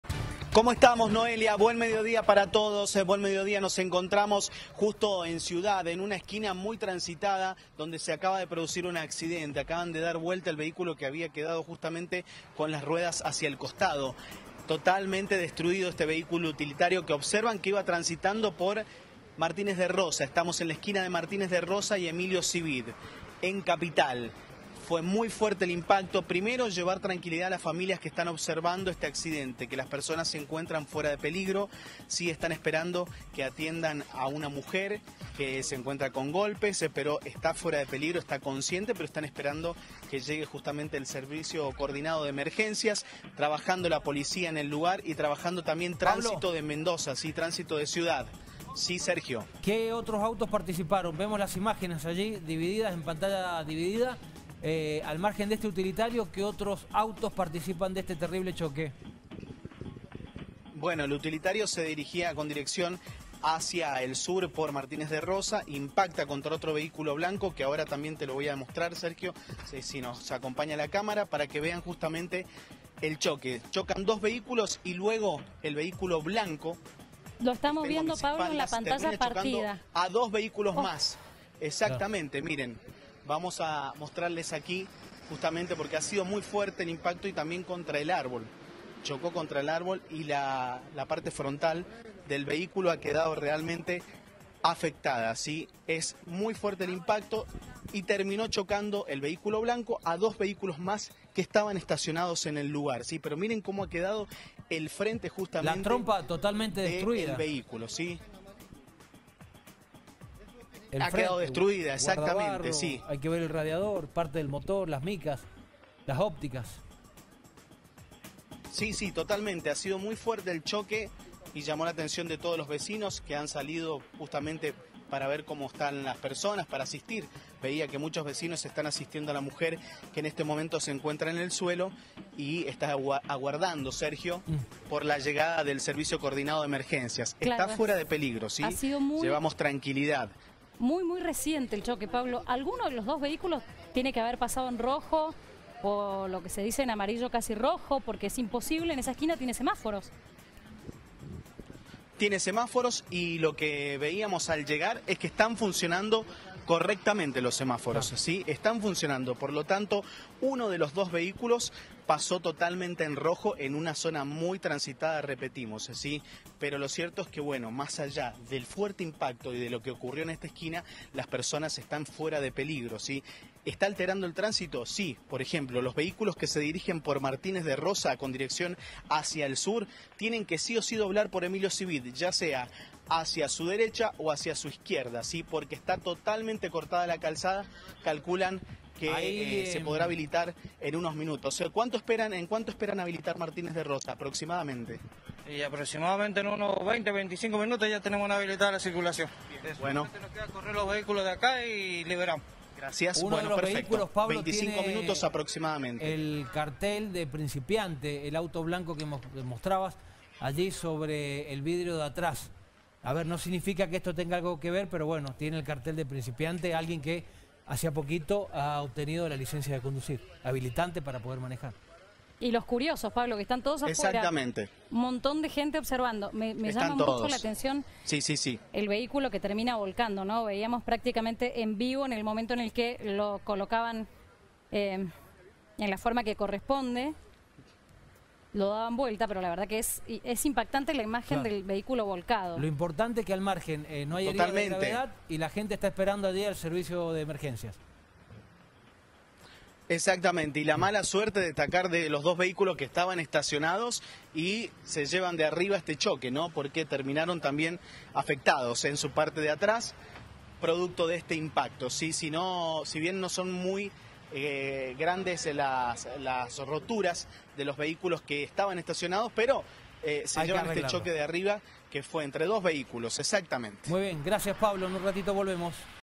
😌CHOCARON 4 AUTOS en CIUDAD
Cuatro vehículos protagonizaron un choque en Quinta Sección (esquina calle Martínez de Rozas y Emilio Civit). Las personas involucradas se encuentran fuera de peligro.